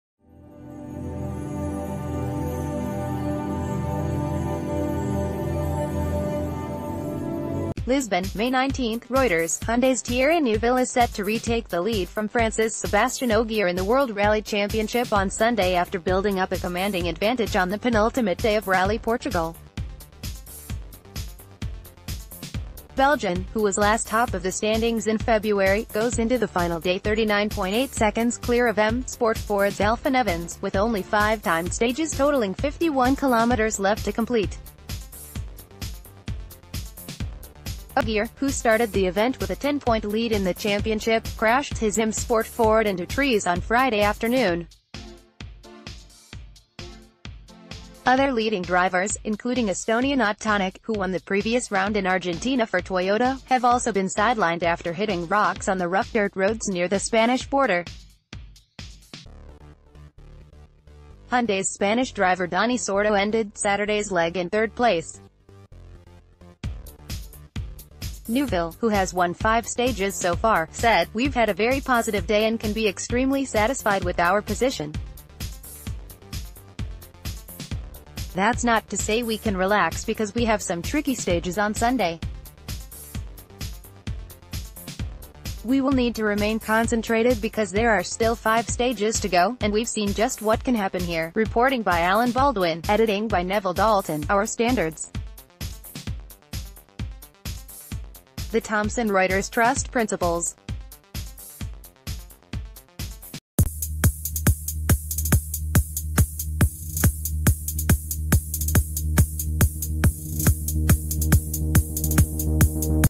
Breaking News | Rallying-Neuville on course for Portugal victory and overall lead
LISBON, May 19 (Reuters) - Hyundai’s Thierry Neuville is set to retake the lead from France’s Sebastian Ogier in the world rally championship on Sunday after building up a commanding advantage on the penultimate day of Rally Portugal. The Belgian, who was last top of the standings in February, goes into the final day 39.8 seconds clear of M-Sport Ford’s Elfyn Evans, with only five timed stages totalling 51km left to complete. Ogier, who started the event with a 10-point lead in the championship,...